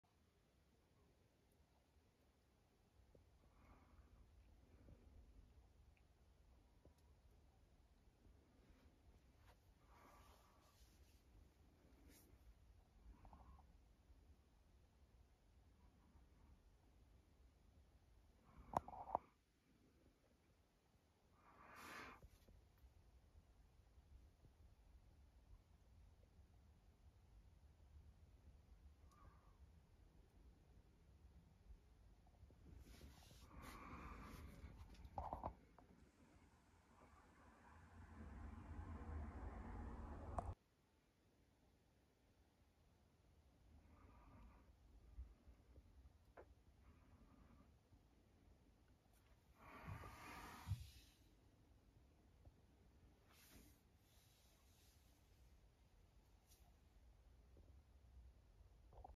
Fireworks for 2024
This is just a little teaser of what I have locked away ready for BFN and NYE. I will be uploading a full stash video when I can.